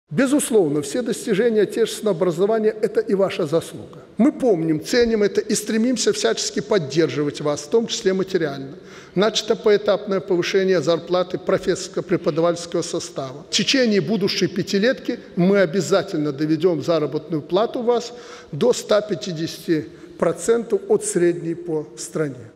Лукашенко: зарплаты профессорско-преподавательского состава доведут до 150% от средней по стране
Зарплаты профессорско-преподавательского состава в течение пятилетки доведут до 150% от средней по стране. Об этом Президент Беларуси Александр Лукашенко заявил сегодня на встрече с педагогическим активом страны.